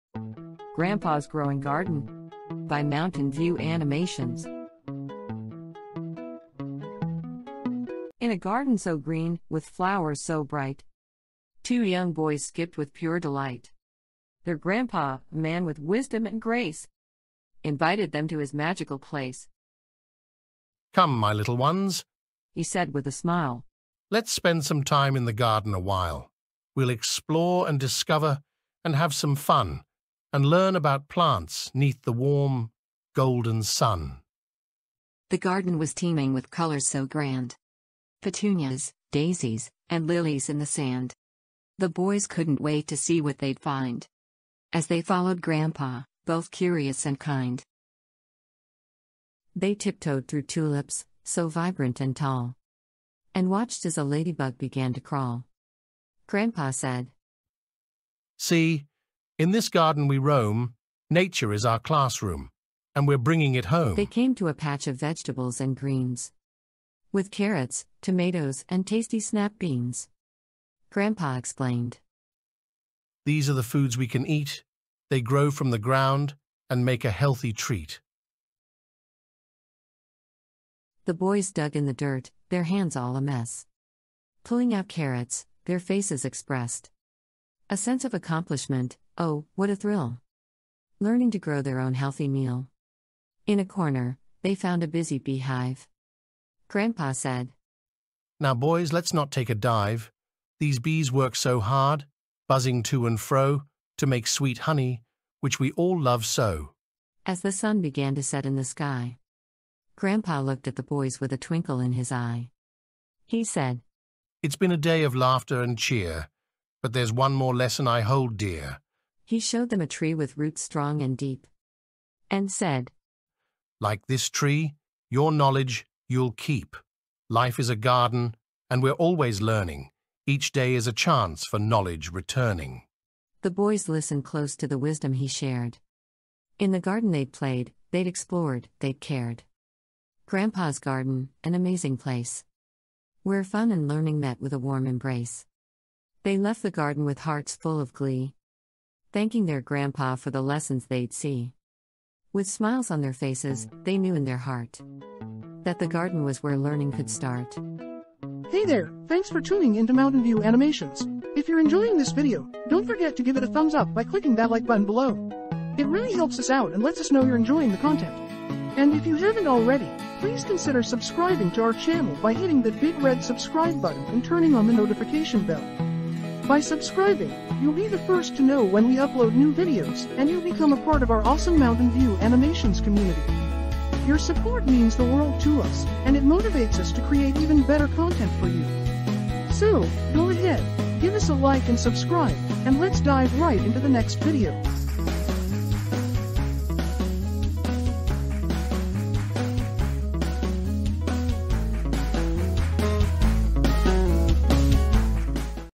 Grandpa's Growing Garden
Grandpa teaches his young grandsons about his garden.